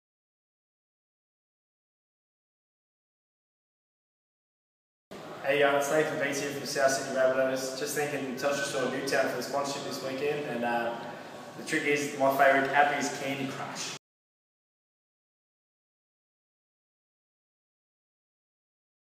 Telstra Newtown| Proud Sponsors SSFC | Meet Nathan Peats
Meet Nathan Peats

Telstra Store Newtown are Proud sponsors of the South Sydney Rabbitohs, Some of the boys will be visiting us in store from time to time!

Nathan is a rugby league footballer who currently plays for the South Sydney Rabbitohs in the NRL. His position of choice is at hooker but he has also played at loose-forward and second row.

Peats was a South Sydney Junior who played for the La Perouse Panthers [2].
Prior to his NRL début, he was Souths' 2010 Toyota Cup captain, scoring 14 tries in 46 NYC games for the Rabbitohs. Peats captained the 2010 NYC South Sydney Rabbitohs team all the way to the Grand Final alongside current Rabbitohs team mates Adam Reynolds, Dylan Farrell and Josh Starling, where the South Sydney Rabbitohs lost to the New Zealand Warriors 42-28.

On 1 April 2011, Peats made his first grade début for the South Sydney Rabbitohs in Round 4 of the 2011 NRL season against the Manly Sea Eagles at Bluetongue Stadium.

Peats played in the Indigenous All Stars in 2012 and 2013, and represented City Origin in 2013.